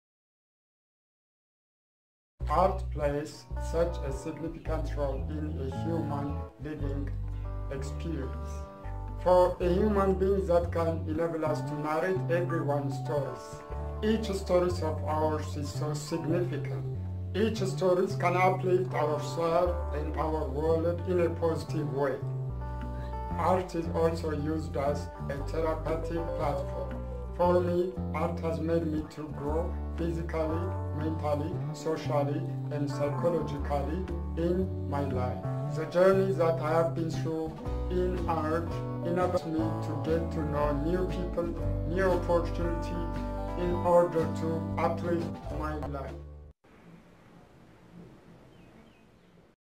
BROOK YESHITILA | ArtEN | Art: an Essential Need
Art: an Essential Need (ArtEN) is a crowdfunded Art Initiative for supporting artists living in Africa.